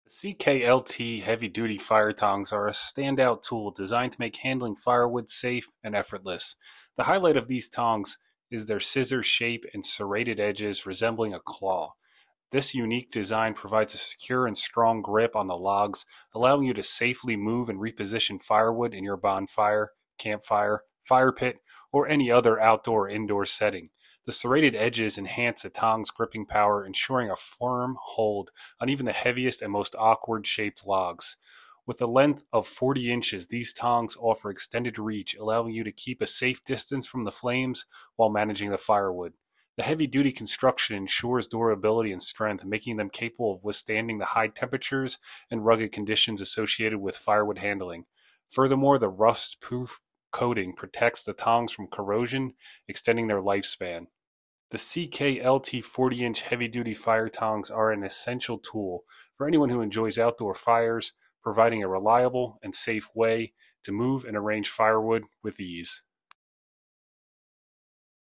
Review: CKLT 40" Heavy Duty Fire Tongs Firewood Grabber Tool with Scissor Shape
CKLT 40" Heavy Duty Fire Tongs Firewood Grabber Tool with Scissor Shape Serrated, Log Claw Tongs for Bonfire Campfire Fire Pit Outdoor Indoor Log Grabber Tongs Rustproof Safely Moves Firewood